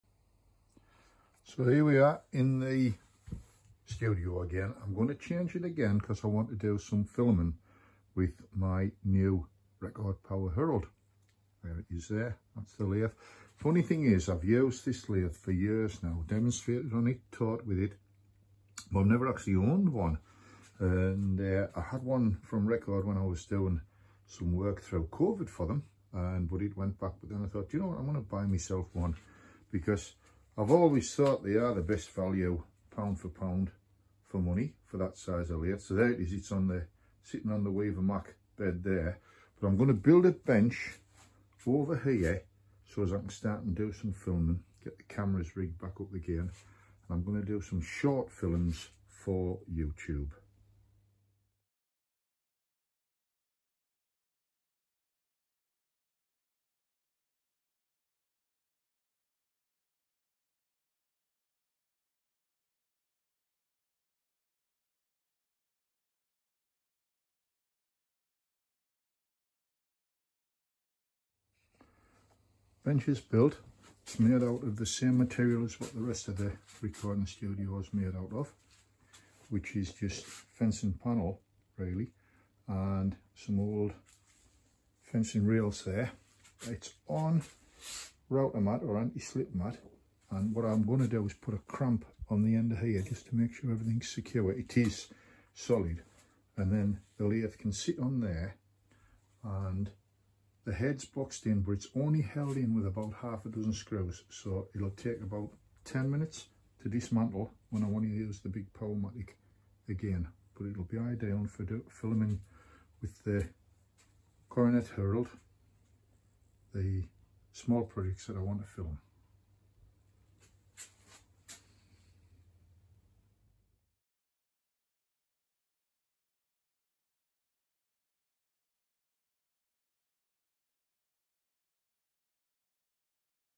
Andrew Hall Woodturner and his new Record Power Coronet Herald
During covid I produced a number of videos for Record Power using the Coronet Herald. I decided to buy the last one which was the show room model from Snainton woodturning a lovely family business Ive known for over 20 years with the view to making some short videos using the Herald.